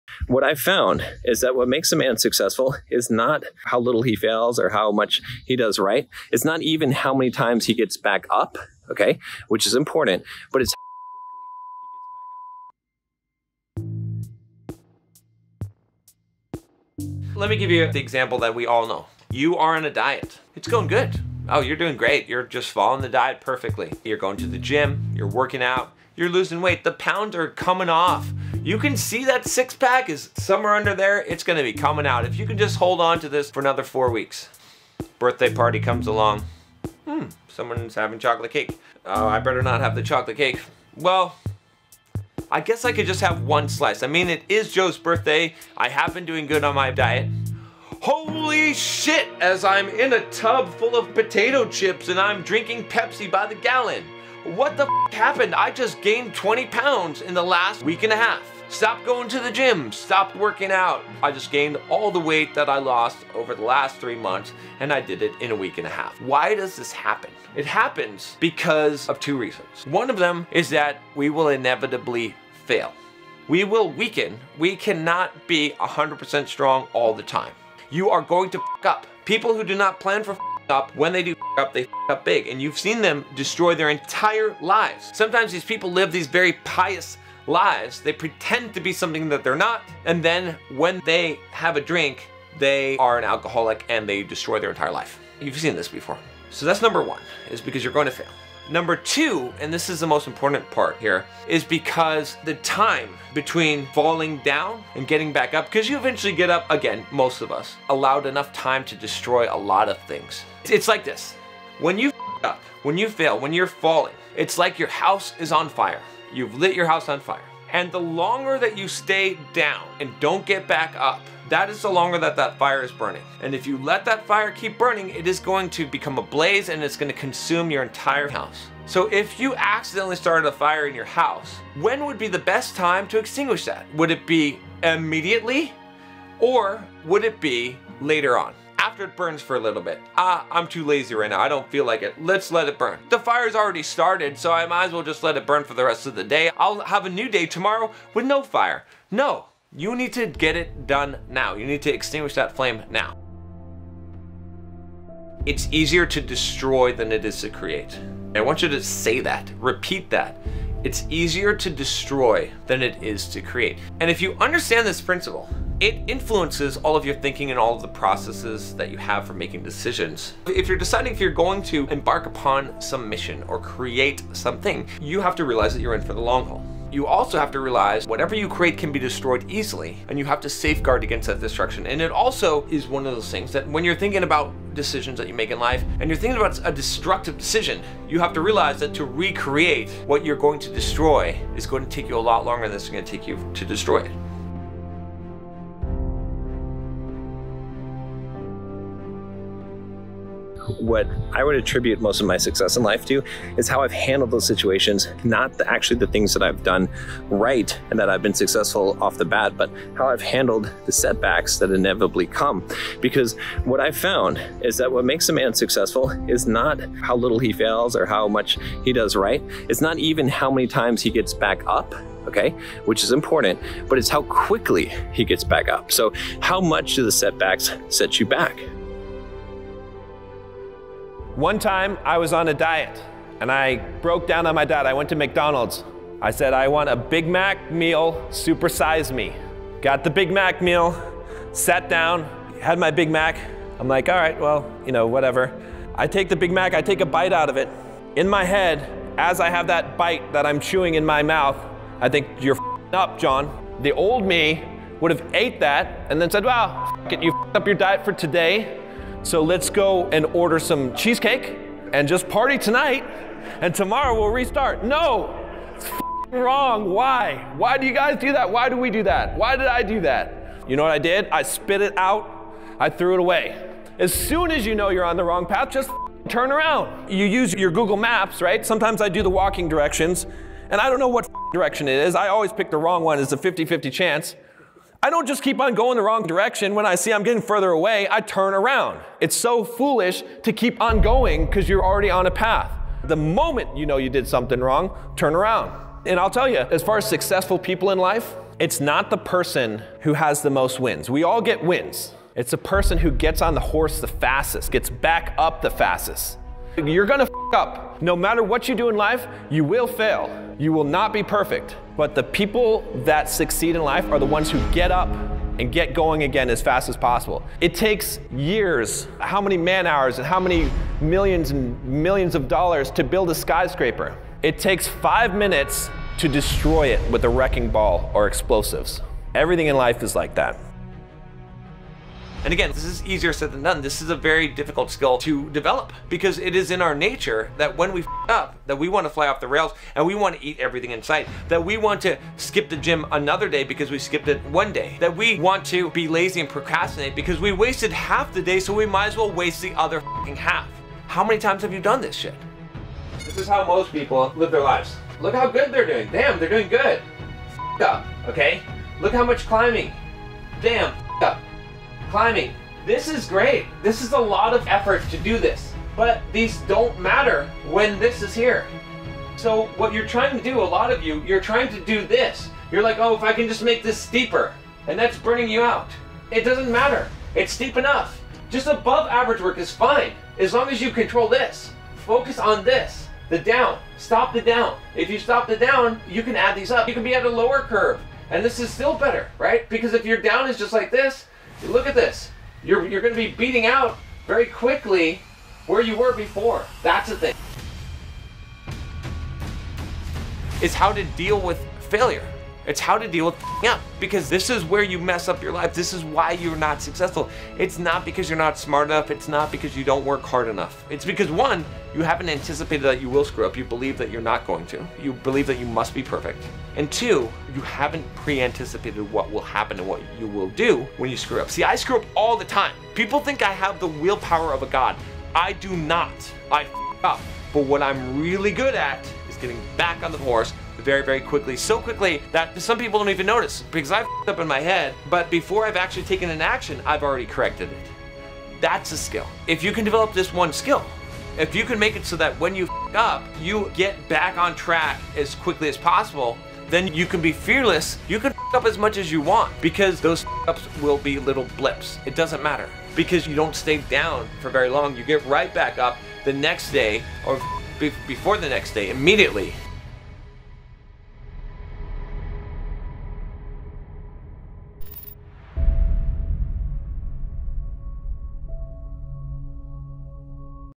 Why you work hard yet still get NOWHERE in life
In this highlight video, John Sonmez from @BulldogMindsetArchive reveals the most important HIDDEN skill every industry giant and successful person masters that you (likely) haven't.

🖊️ Notable quotes:
1:11
"We will INEVITABLY fail [at some point]."

1:19
"People who do not plan for f*cking up, when they do f*ck up, they f*ck up BIG."

2:40
"It's easier to destroy than it is to create."

3:34
"What makes a man successful is not how little he fails or how much he does right. It's not even how many times he gets back up (which is important), but it's how QUICKLY he gets back up."

4:52
"The MOMENT you know you did something wrong, TURN AROUND!"